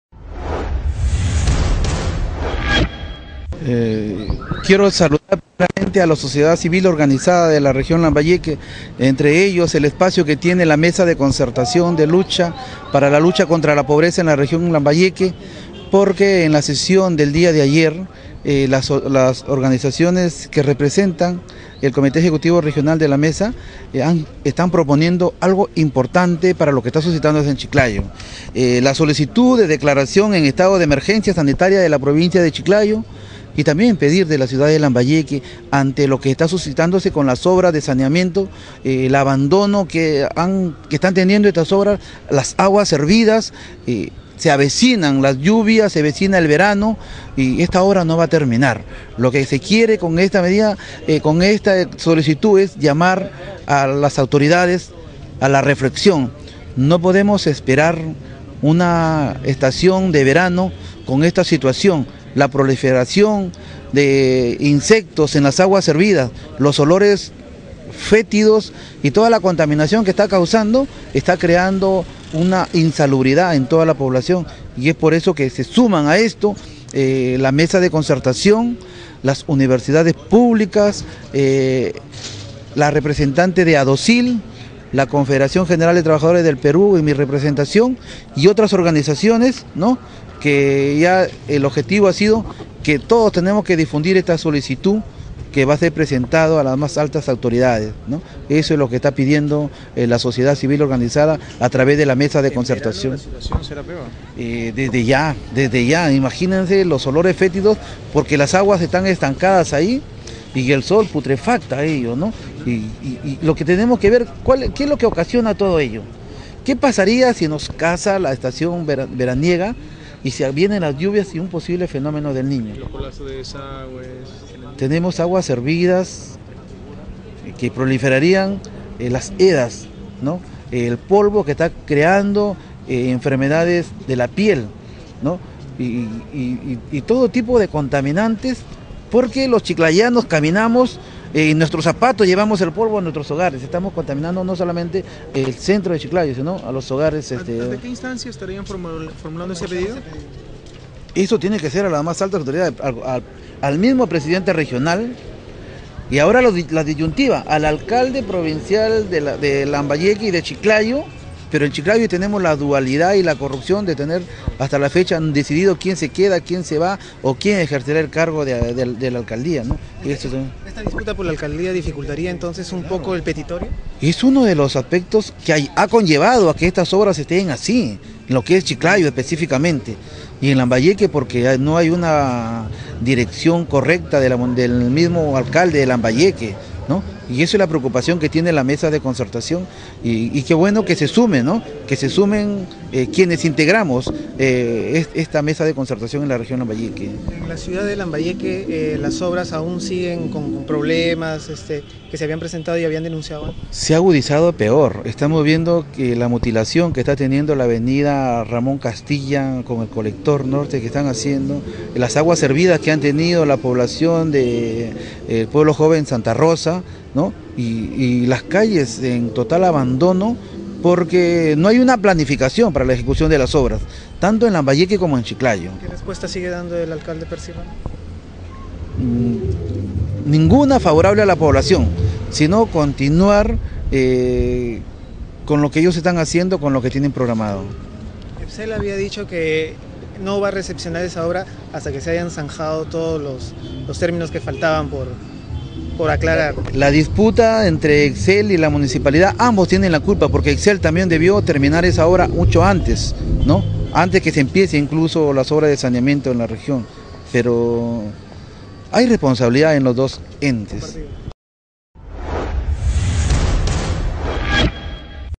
DENUNCIA WILMER ANTON
MCLCP Y SOCIEDAD CIVIL SOLICITA SE DECLARE A CHICLAYO EN ESTADO DE EMERGENCIA SANITARIA